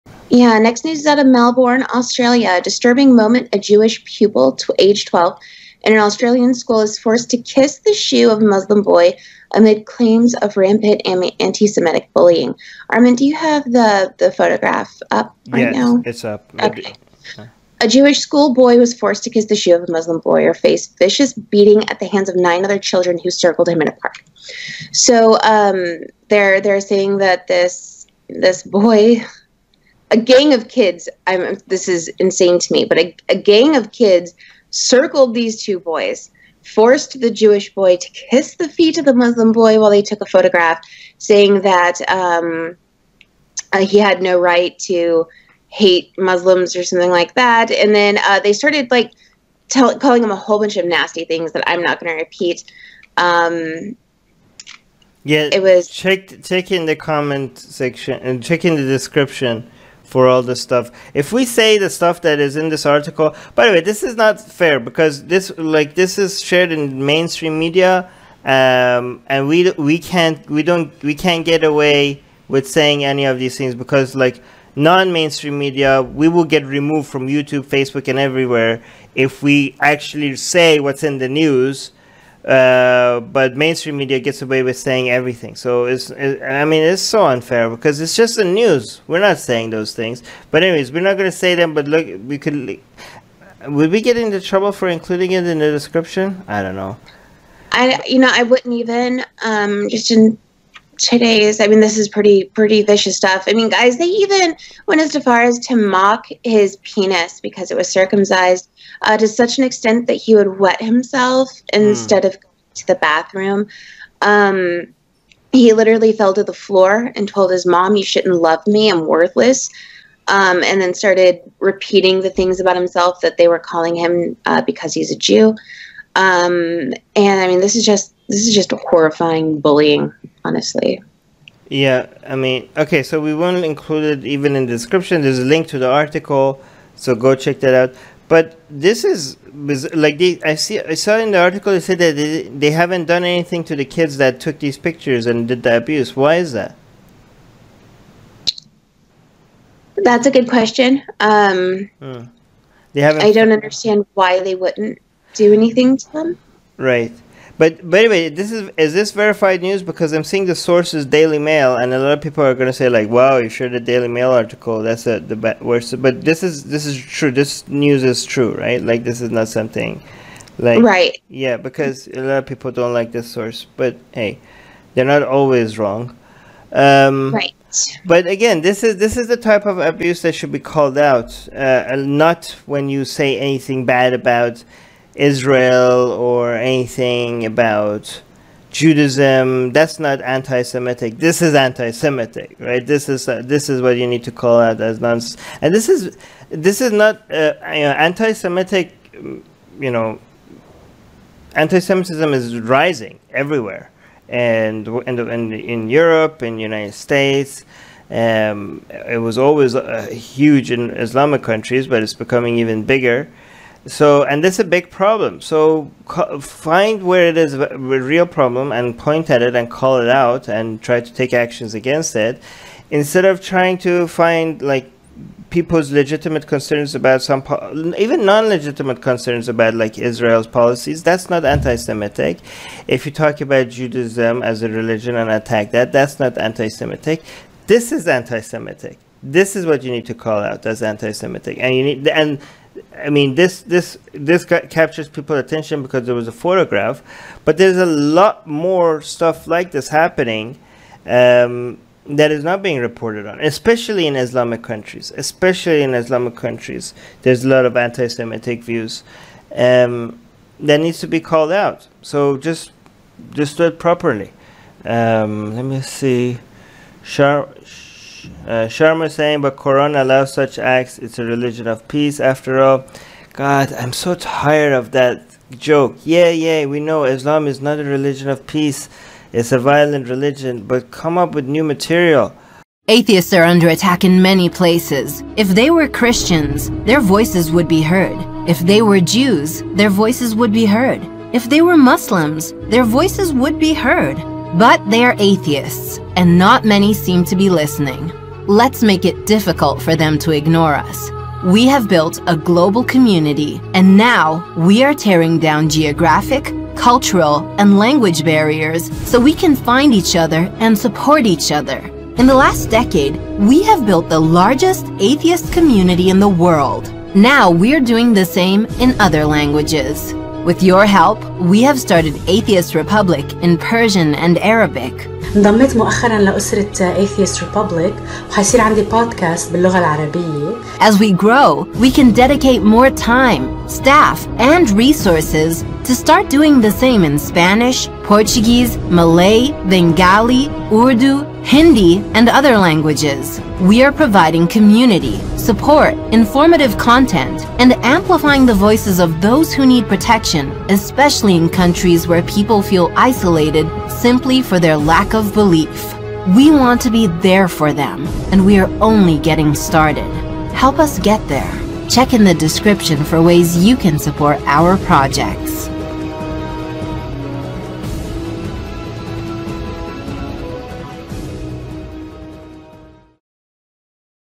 Jewish Pupil Is Forced to Kiss the Shoe of a Muslim Boy ✡️
A Jewish schoolboy was forced to kiss the shoe of a Muslim boy or face a vicious beating at the hands of nine other children who circled him in a park. A photograph shows a 12-year-old on his knees kissing the shoe of a Muslim boy, allegedly after a group 12 and 13-year-old's threatened him with violence. It is unclear whether the Muslim boy was forced to pose for the photo by the gang, who have so far escaped disciplinary action, The Age reported. In another instance of anti-Semitic abuse in a Melbourne school, a five-year-old boy was called a 'Jewish cockroach' and 'dirty Jew' by his classmates, who mocked his circumsized penis to such an extent that wet himself instead of using the toilet. The mother of the younger boy also revealed her heartbreak at the abuse her son suffered. 'He literally fell down on the floor and said, ''Mummy, you shouldn't love me. I'm a worthless, Jewish rodent. I'm vermin'',' she told the Australian Jewish News.

Source: Disturbing moment a Jewish pupil, 12, at an Australian school is forced to KISS the shoe of a Muslim boy amid claims of rampant anti-Semitic bullying
Location: Melbourne, Australia